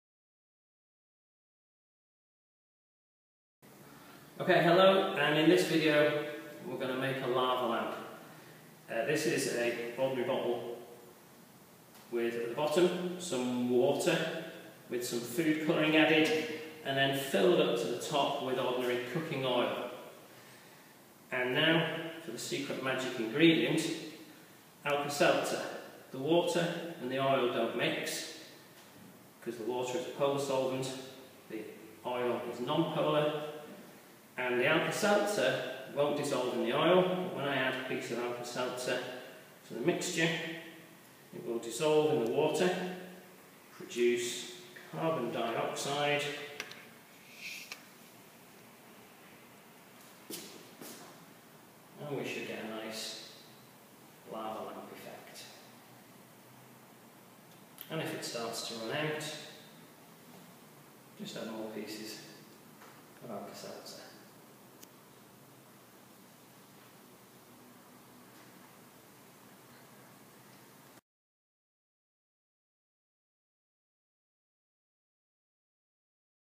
How to make a Lava Lamp - Liverpool Community College
Adrian Greenall from the Science Department at Liverpool Community College shows us how to make a Lava Lamp.